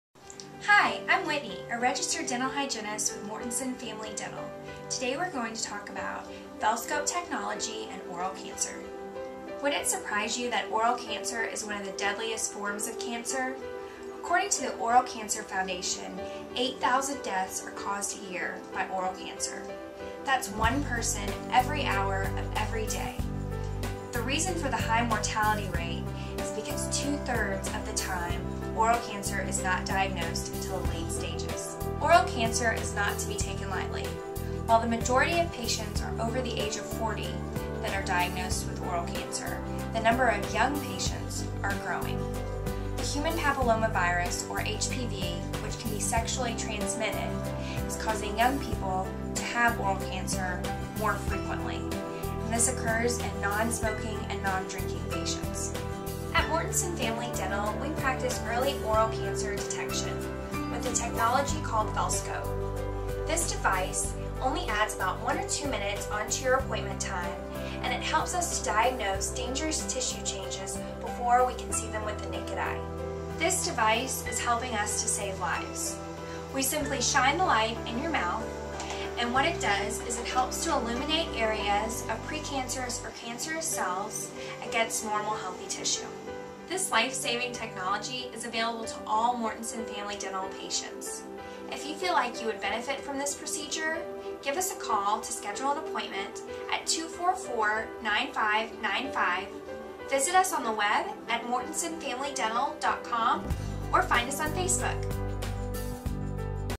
We're Saving Lives With VELscope Technology
Did you know that oral cancer is one of the deadliest forms of cancer?  Watch this video to find out more!